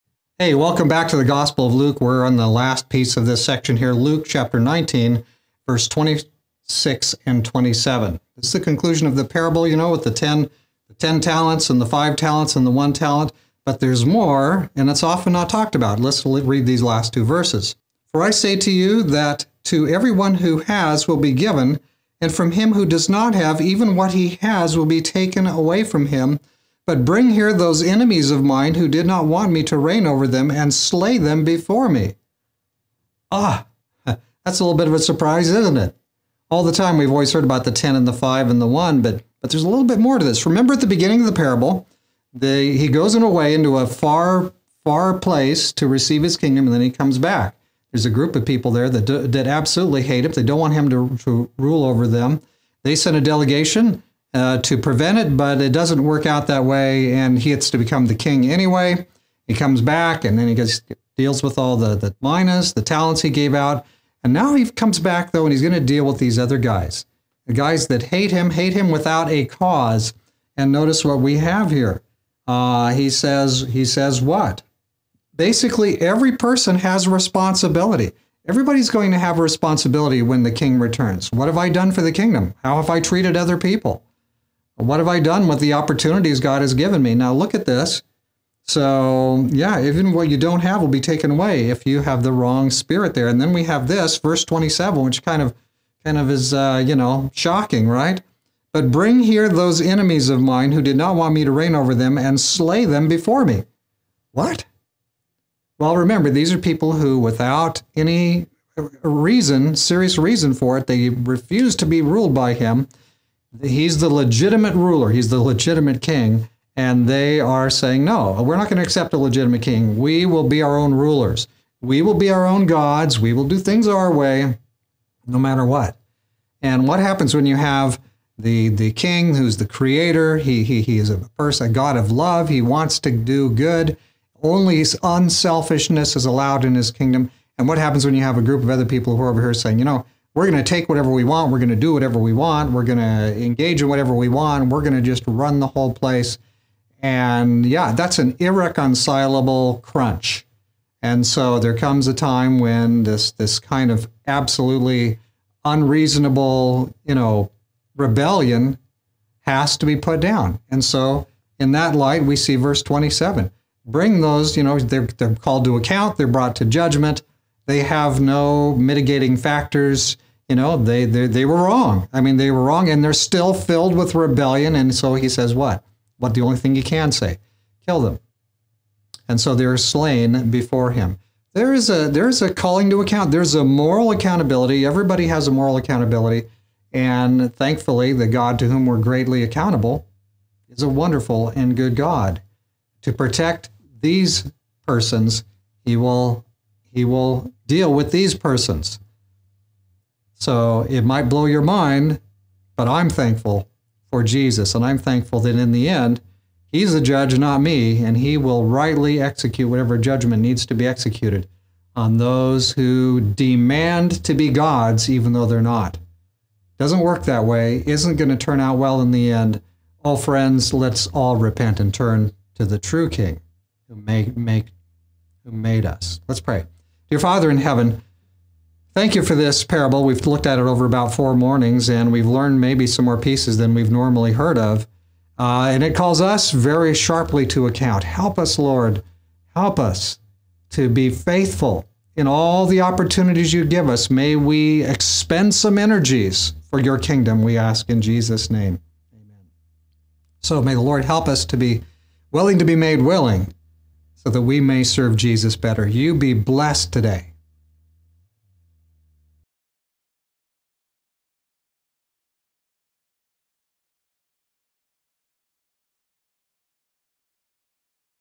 Luke 19:24-27 Return of the King 4
In the end there is an accounting. Let us use our privileges and responsibilities now well.